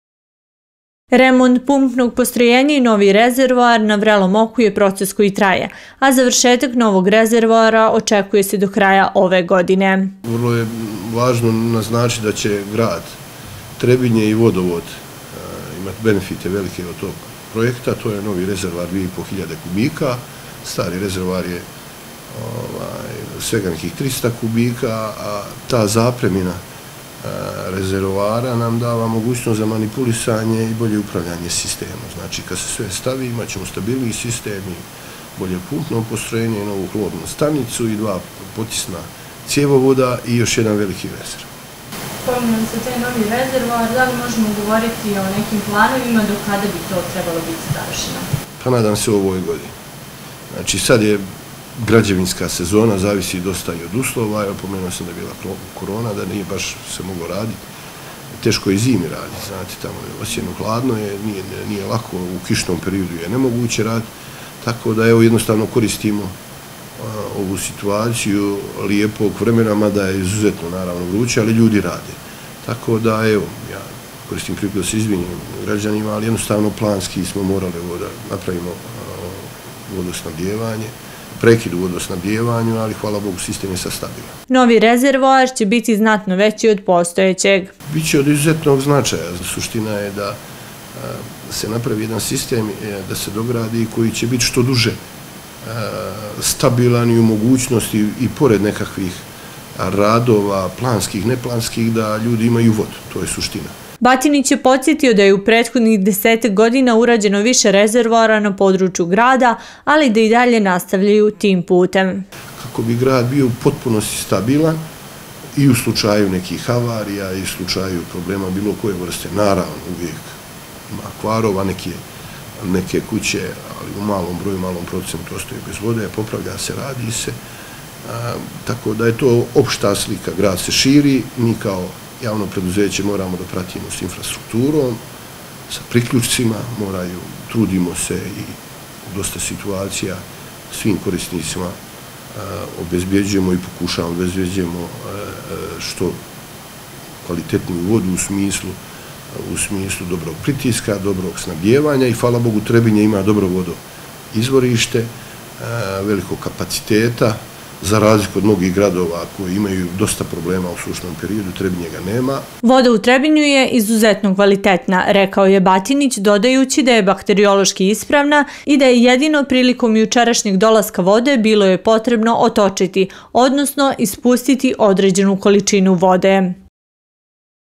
Trebinje: Voda ispravna za piće, novi rezervoar do kraja godine
U  Trebinju je zbog remonta pumpnog postrojenja u noći između 13. na 14. jul došlo do najavljenog nestanka vodosnadbijevanja. Voda je kod svih potrošača stigla juče i ispravna je za piće. Inače remont pumpnog postrojenja je planski posao koji se sprovodi shodno vodoprivrednoj dozvoli HET-a.